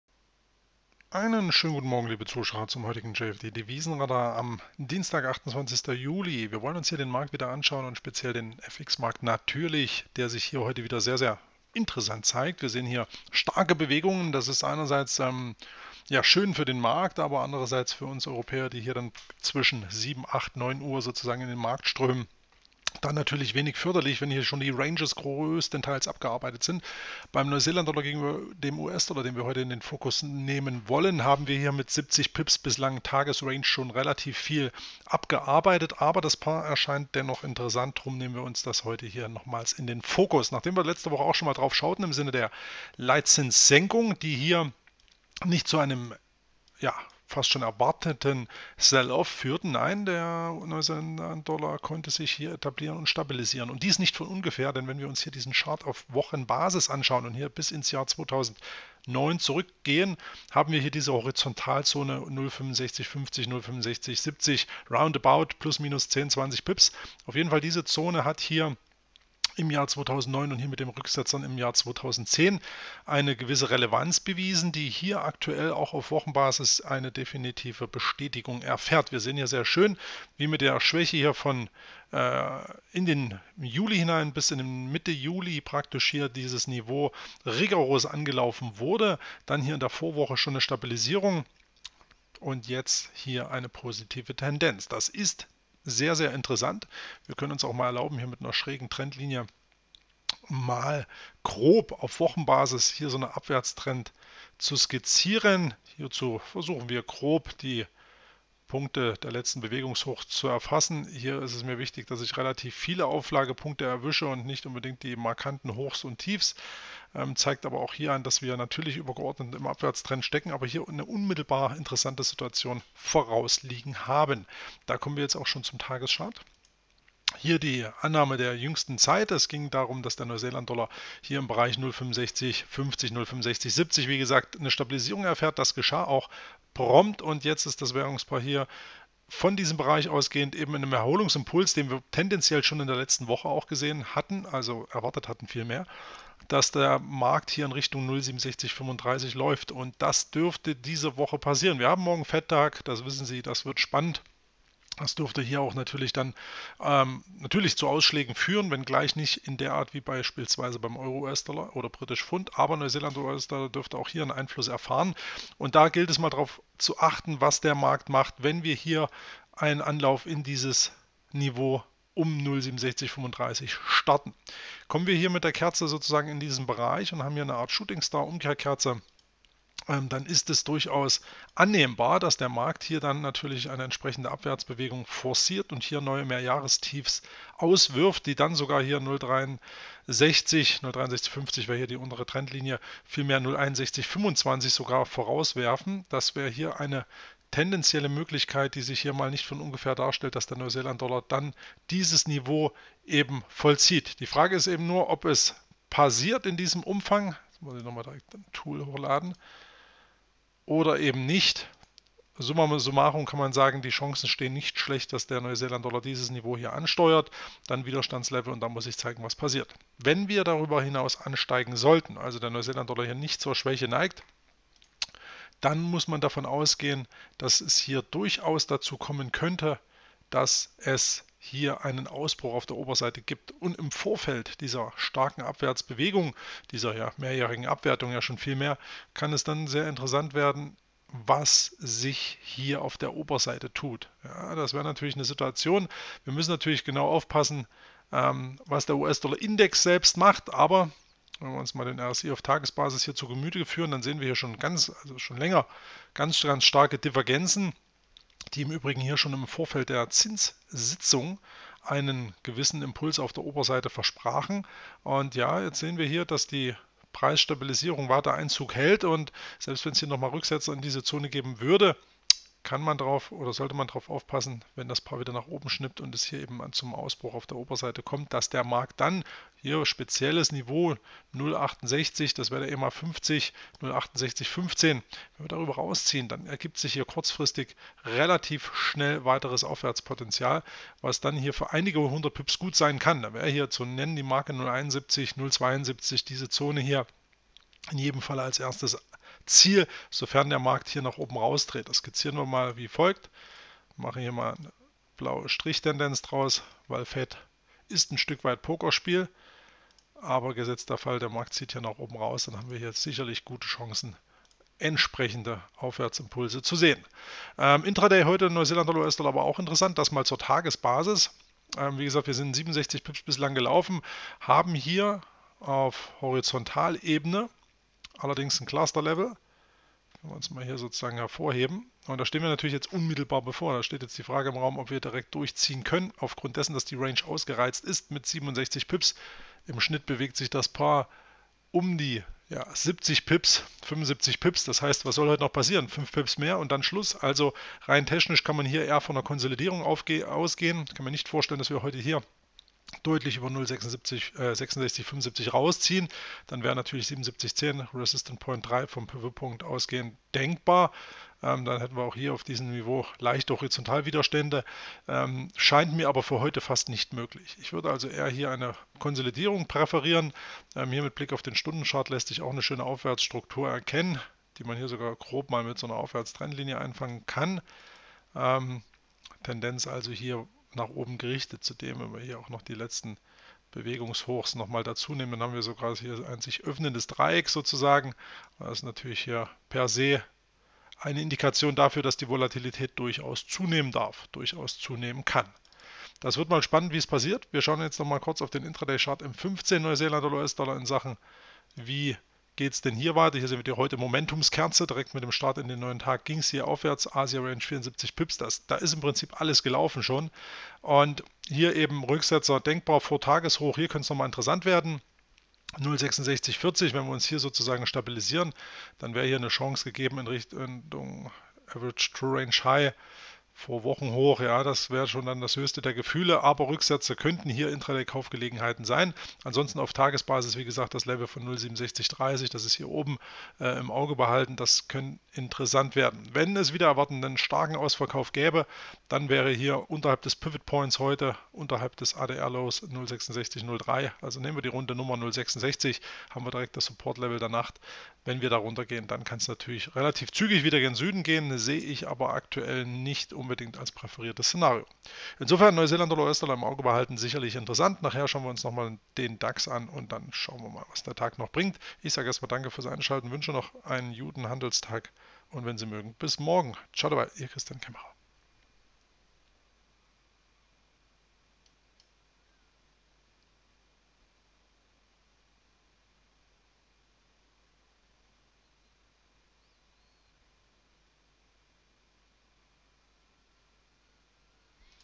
JFD Devisenradar: NZD/USD bleibt unverändert interessant
Der Kiwi zeigt sich weiterhin stabilisiert. Doch wie lange noch? Der Chart gibt Aufschluss!